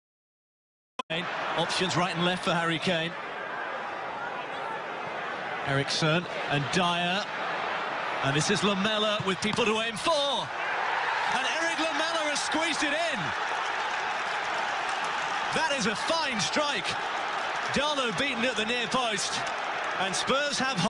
Lamela
El gol de Erik Lamela en la derrota del Tottenham frente al Newcastle.